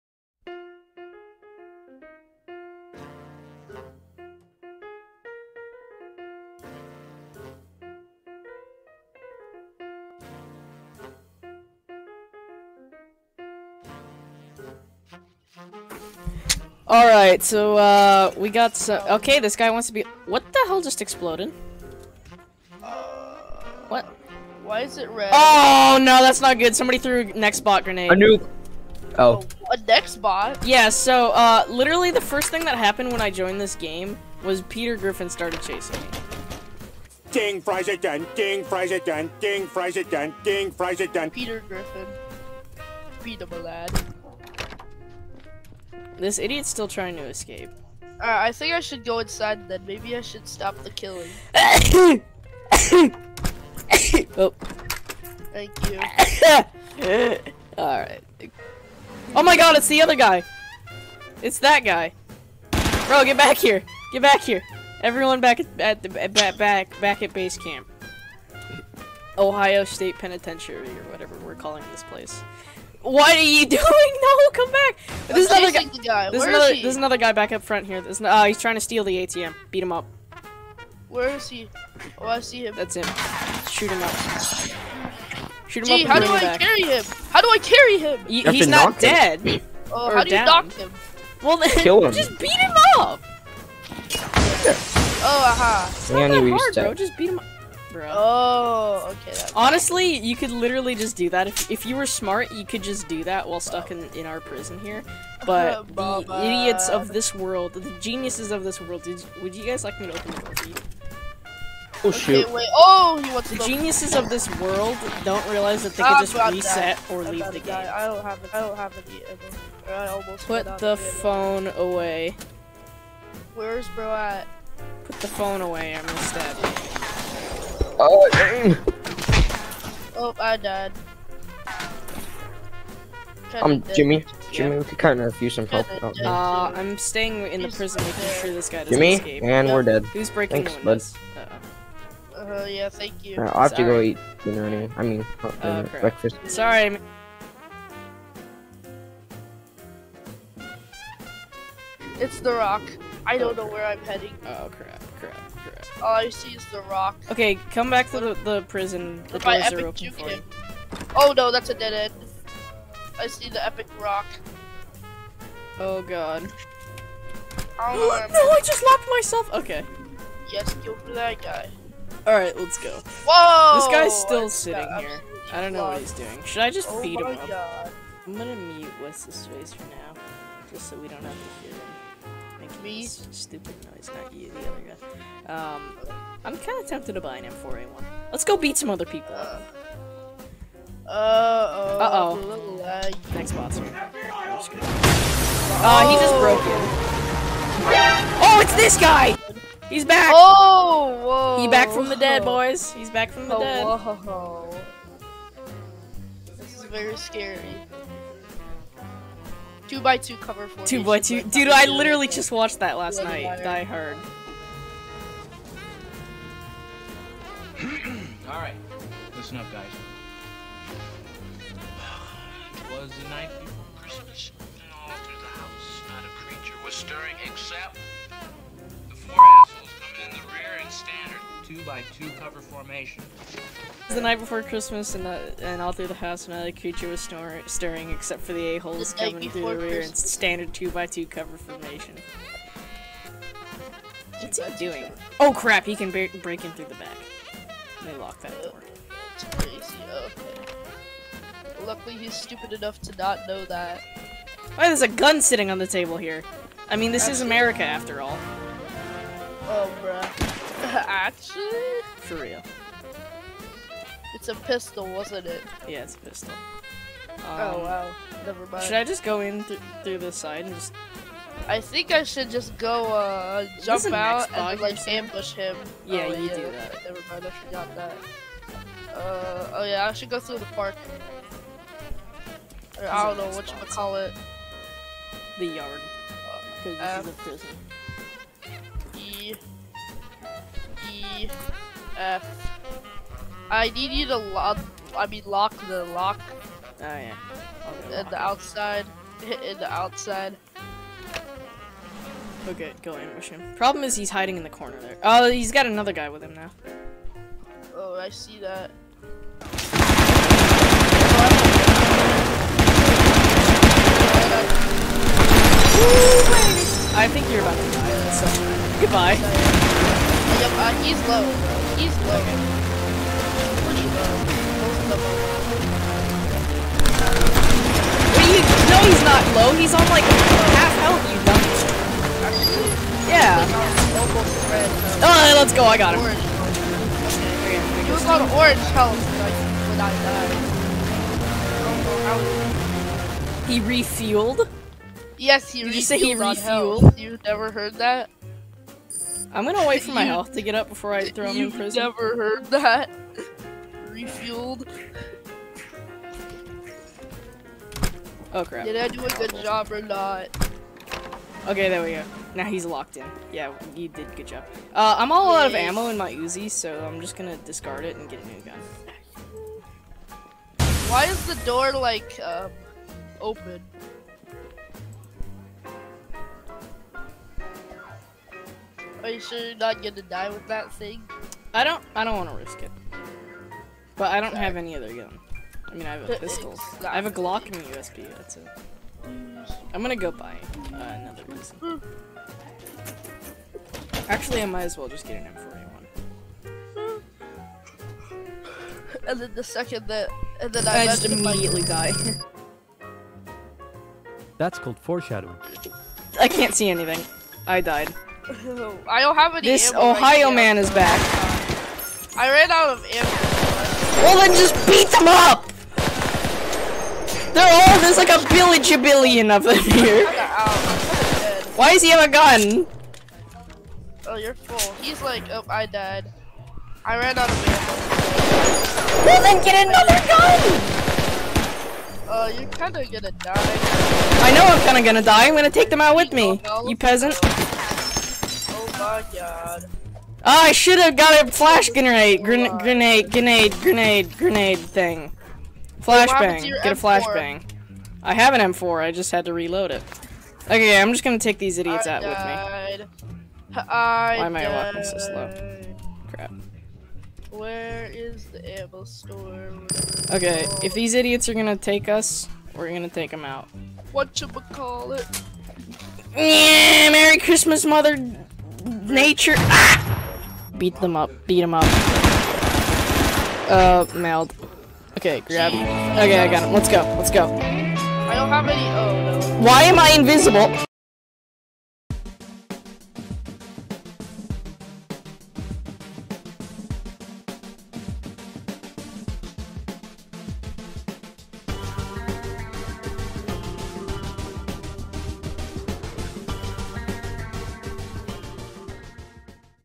I sent people to prison in Ohio | Part 1
Greetings peasants, today you can watch me be an absolute idiot with ltz_Dame as we try to lock people in prison in Ohio. Since Ohio has become the thing of memes recently, I've decided to jump on the bandwagon and get some content before the Ohio memes die out.